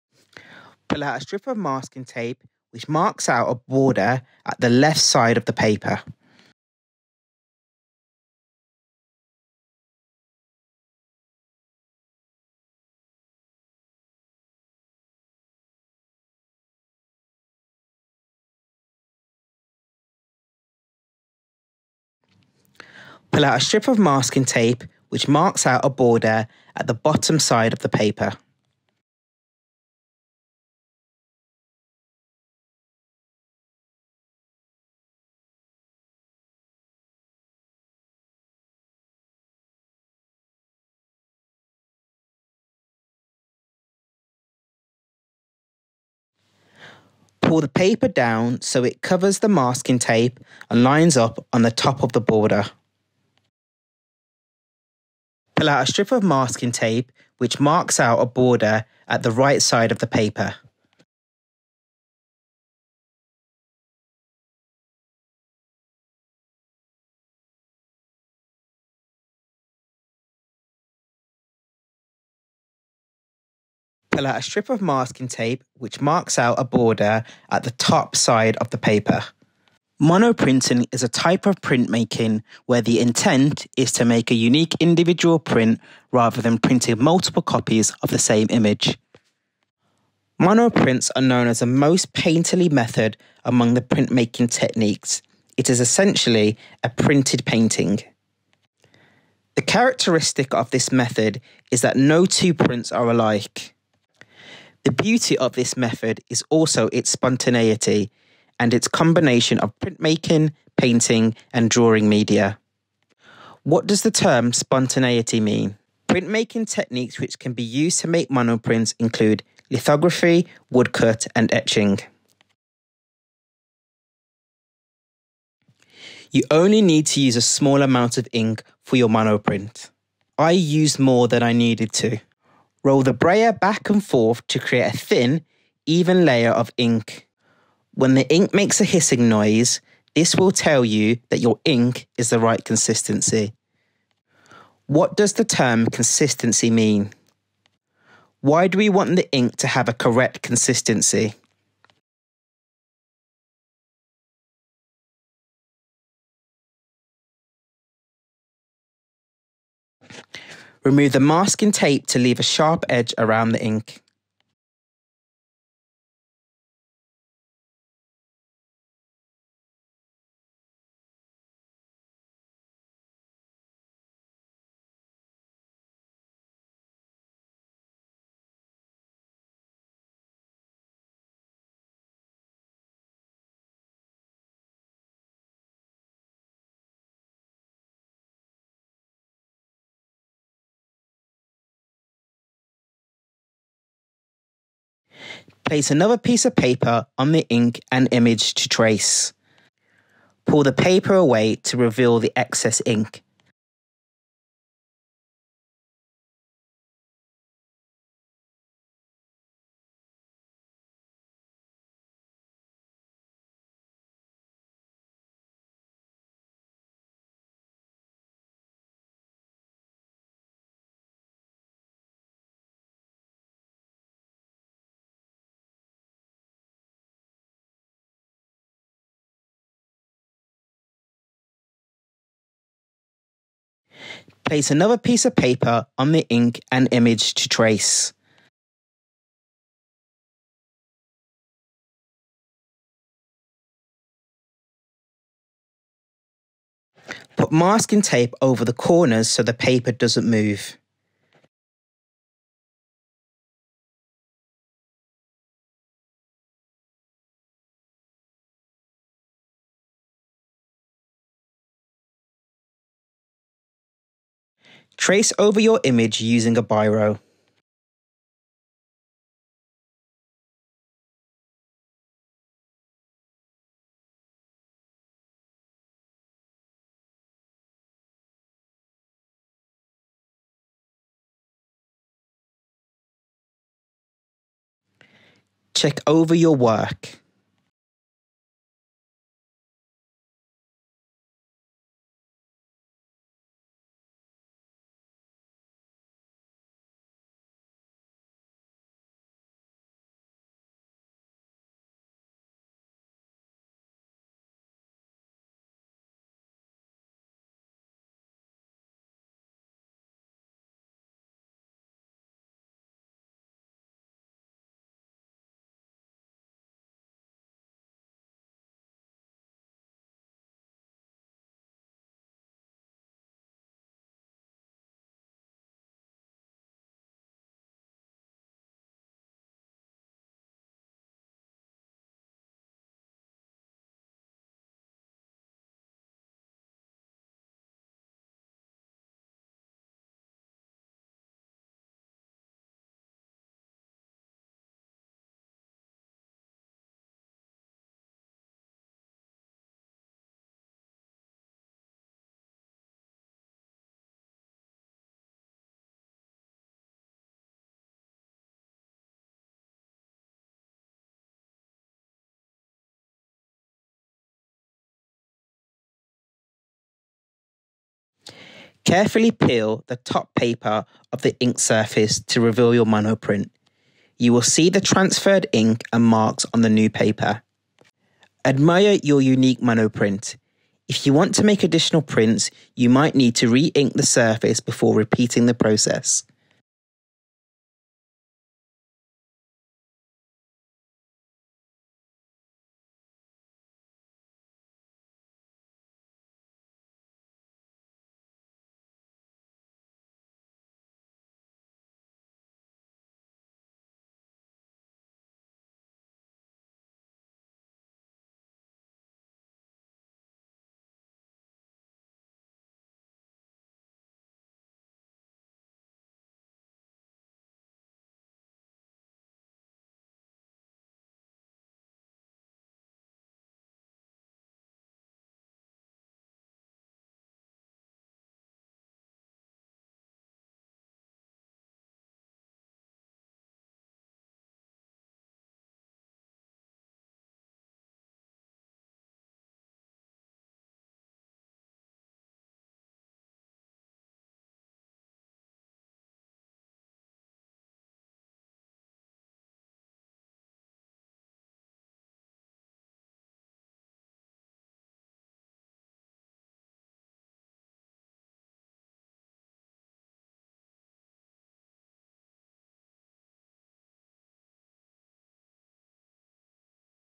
How To Make A Patrick Caulfield Inspired Mono Print
Mono-printing is a type of printmaking where the intent is to make a unique individual print rather than printing multiple copies of the same image.

By doing this you will know where to spread your ink.

Using masking tape create a border around the side and bottom of your image.

Pull out a strip of masking tape which marks out a border at the right side of the paper.

Pull out a strip of masking tape which marks out a border at the top side of the paper.

Pull out a strip of masking tape which marks out a border at the left side of the paper.

Pull out a strip of masking tape which marks out a border at the bottom side of the paper.

Pull the paper down so that it covers the masking tape and lines up on top of the border.

You only need a small amount of ink for your mono-print.

I used more than I needed to.

When the ink makes a hissing noise, this will tell you that your ink is the right consistency.

What does the term consistency mean?

Why do we want the ink to have a correct consistency?

Roll the brayer back and forth to create a thin, even layer of ink on the paper. This will serve as your printing surface.

Remove the masking tape to leave a sharp edge around the ink.

Pull piece of paper over the ink and pat it gently with your hands.

Pull the paper away to reveal the excess ink.

Place another piece of paper on the ink and image to trace.

Put masking tape over the top corners so the paper doesn't move.

Trace over your image using a Biro.

Work over every part of the photograph.

Check over your work.

Whilst you draw, attempt to apply little pressure on the paper with you hand and wrist as ink sticks to the paper easily.

Monoprints are known as the most painterly method among the printmaking techniques; it is essentially a printed painting.

The characteristic of this method is that no two prints are alike.

The beauty of this method is also in its spontaneity, and it is a combination of printmaking, painting and drawing media.

What does the term spontaneity mean?

Printmaking techniques which can be used to make mono-prints include lithography, woodcut, and etching.

Carefully place a sheet of paper onto the paint-covered plate. Gently press it down to ensure even contact between the paper and the plate.

Carefully peel the top paper off the inked surface to reveal your mono-print. You will see the transferred ink and marks on the new paper.

Admire your unique mono-print! If you want to make additional prints, you might need to re-ink the surface before repeating the process.

Decide on the concept or design you want for your mono-print. You can work from imagination, reference images, or sketches